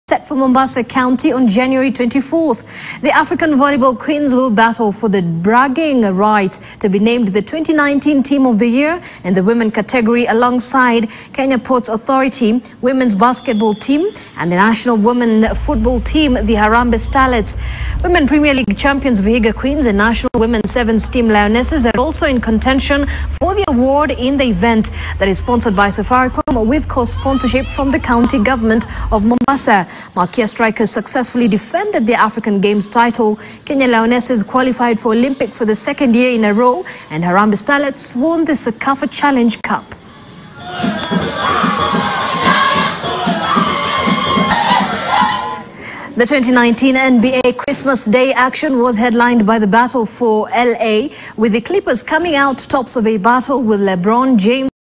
SOYA Awards: Malkia, Starlets, Lionesses to battle for 2019 team award
The national women's volleyball team, the Malkia Strikers, are among the five finalists who will be battling for the top prize during the Safaricom Sports Personality of the Year (SOYA) Gala set for Mombasa County on January 24.